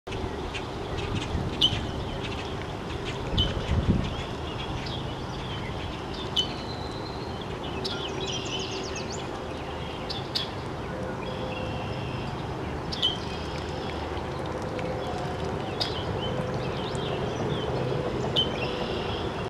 Ducks At the Swamp
A group of ducks with one male, eating in a local swamp on a nice evening.